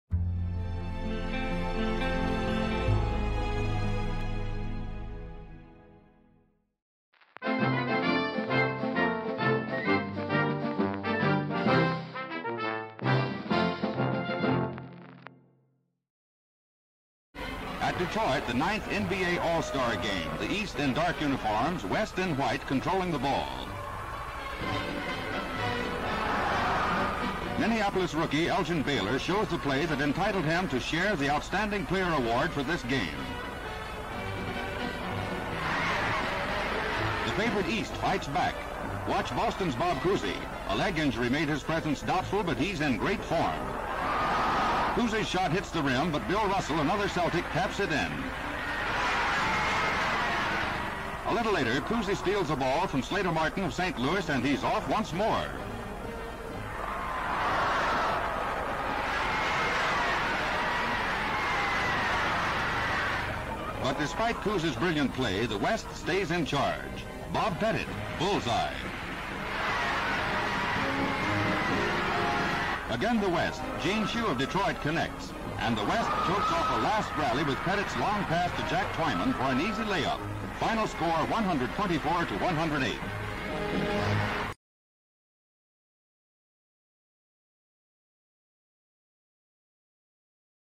1959 NBA All Star Game highlights IN COLOR
Newsreel footage of the 1959 NBA All Star game...presented here IN COLOR

The game, held at Detroit’s Olympia Stadium, featured a first in NBA All-Star history: the sharing of game MVP honors. Rookie Elgin Baylor of the Minneapolis Lakers (24 points, 11 rebounds) and St. Louis Hawks veteran Bob Pettit (25 points, 16 rebounds) split the honors as the West topped the East 124-108

While the award was Pettit’s second straight and third overall, it was one of two awards Baylor won that season (the other being the Rookie of the Year). The West used big runs in the second and fourth quarters to bury the East, which struggled from the field (32.4 FG pct.)

Bob Cousy had perhaps the best game for the East, finishing with 13 points, five rebounds and four assists in the defeat